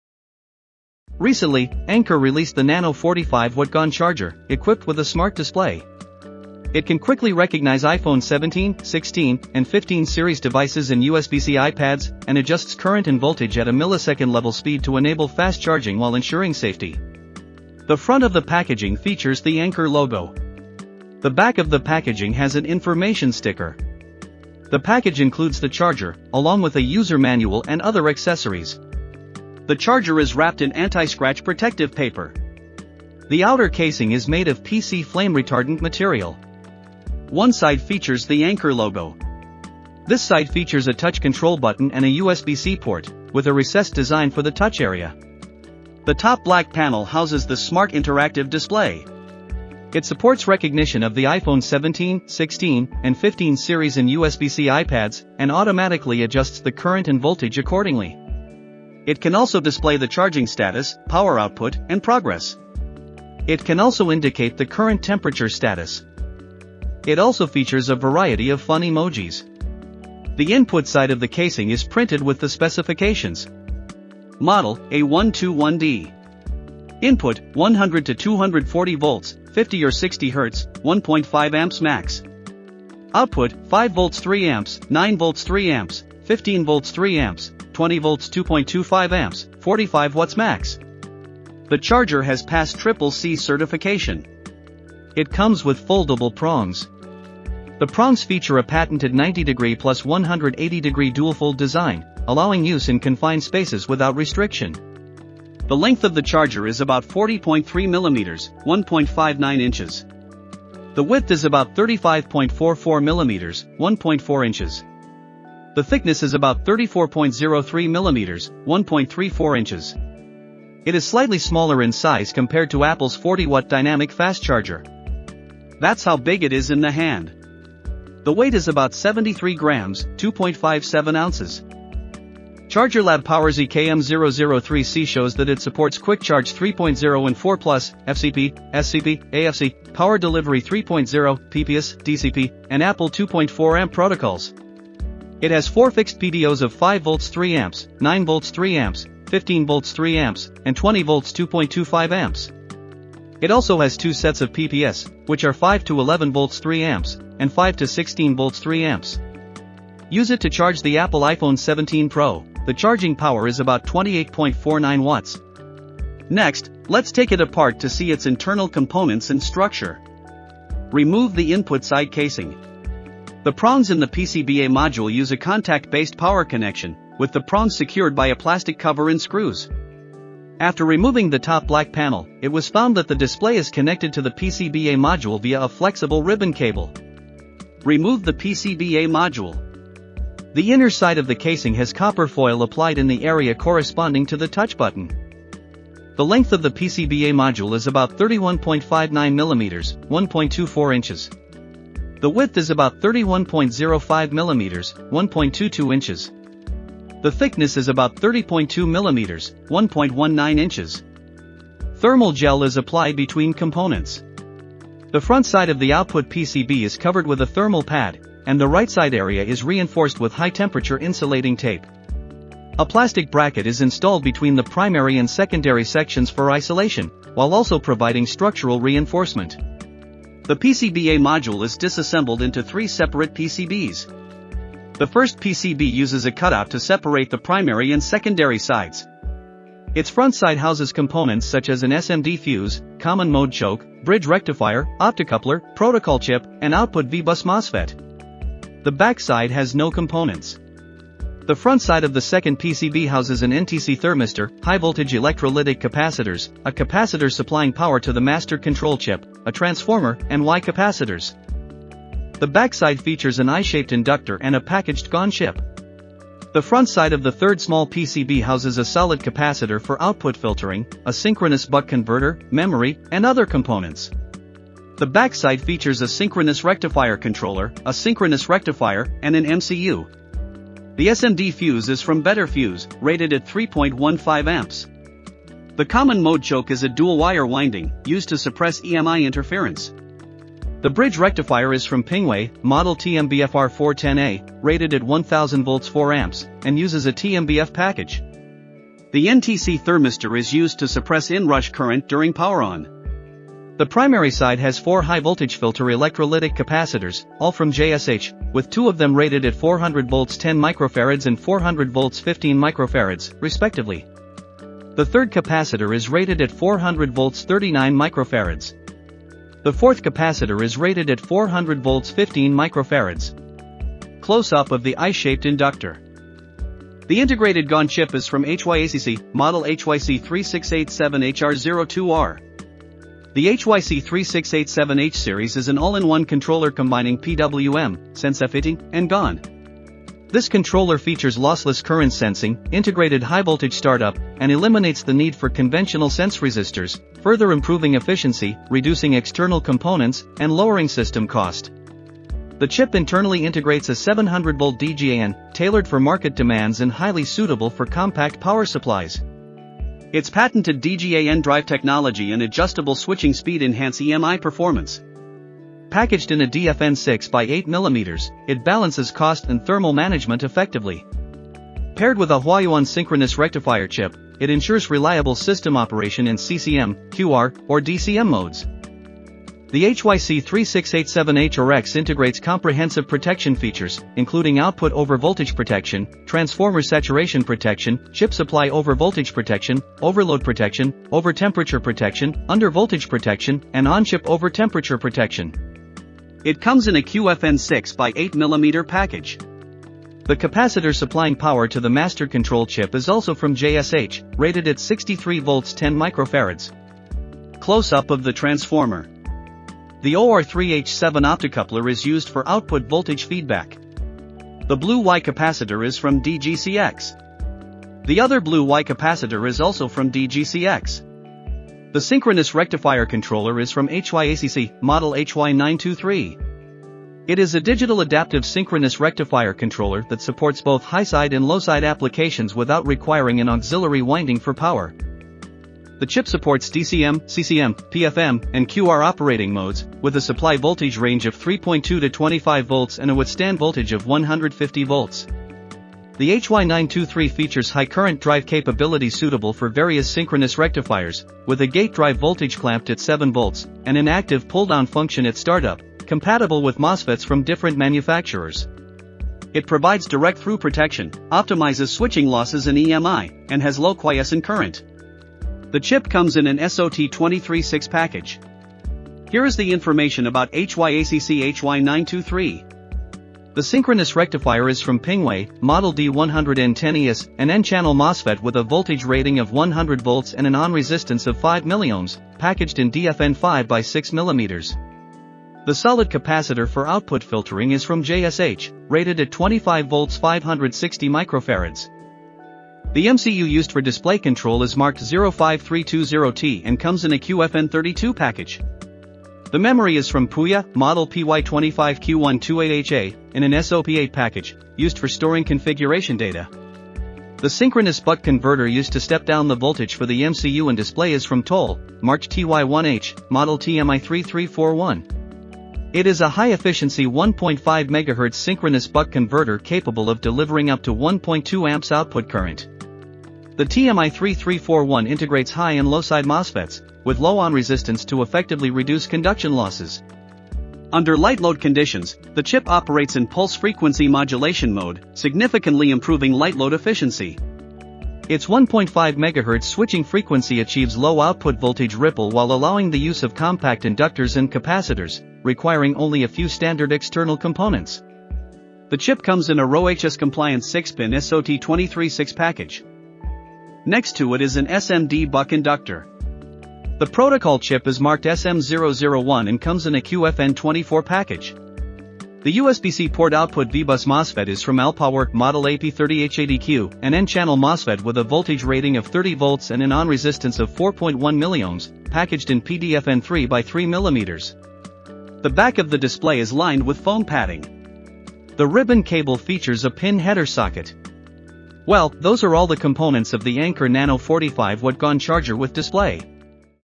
Teardown of Anker Nano 45W GaN Charger with Display (A121D)
Introduction
Recently, Anker released the Nano 45W GaN Charger, equipped with a smart display. It can quickly recognize iPhone 17/16/15 series devices and USB-C iPads, and adjusts current and voltage at a millisecond-level speed to enable fast charging, while ensuring safety.

The display supports various features such as fun emojis, charging power, temperature, and device battery progress bars. Additionally, it offers two modes for fast charging and device protection, which automatically switch based on the charging conditions. Below, we take a closer look at the internal components and design.